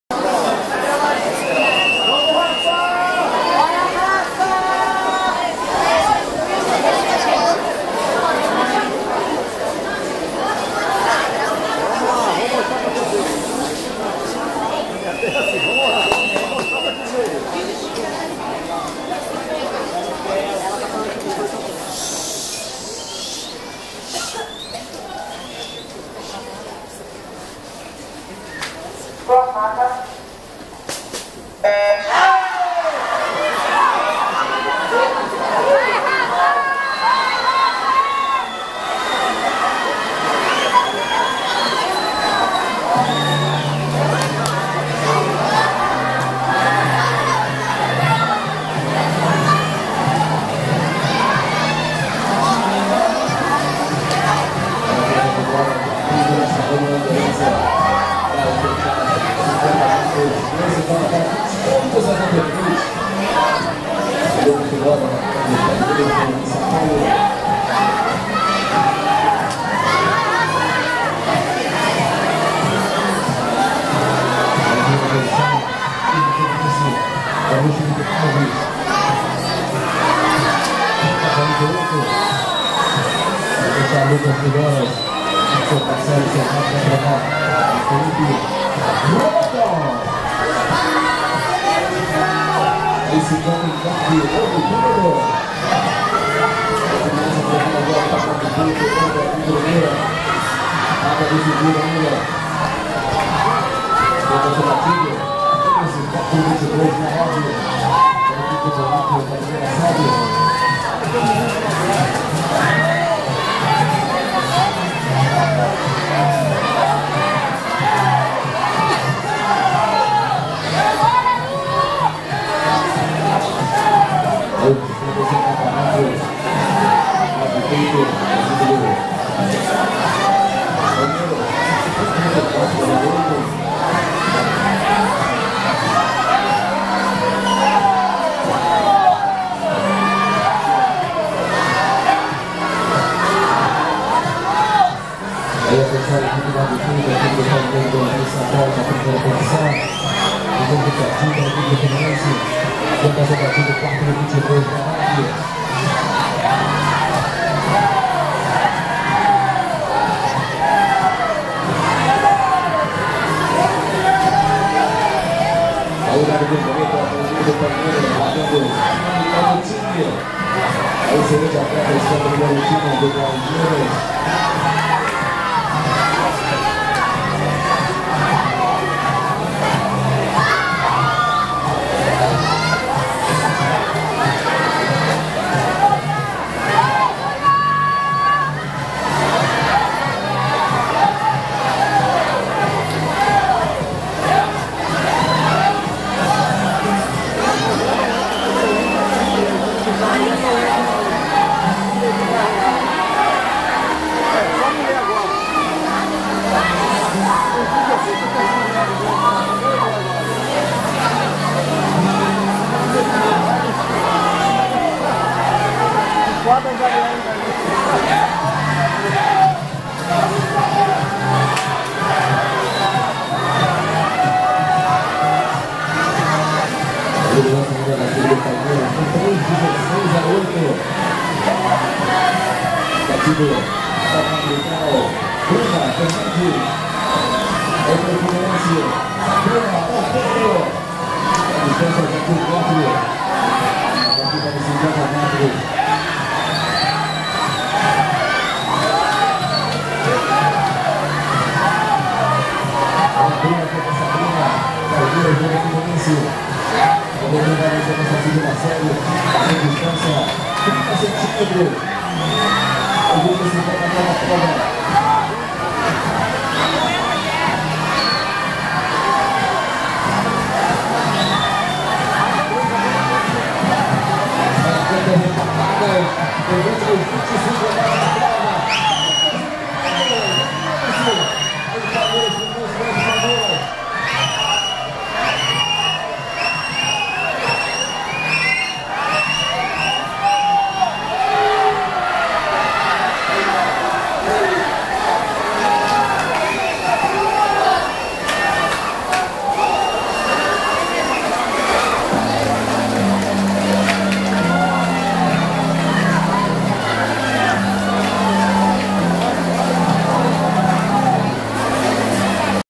Campeonato Brasileiro Infantil de Natação - Rev 4x100 misto
Revezamento 4x100m medley misto infantil no Campeonato brasileiro infantil interclubes de natação de inverno - Troféu Ruben Dinard de Araújo 2018